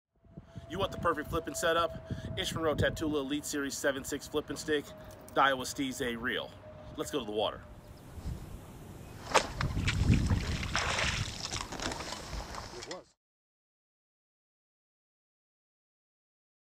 The Best Flippin / Pitching Set Up
Here is a look at Ish Monroe's go to set up got lighter flipping and pitching.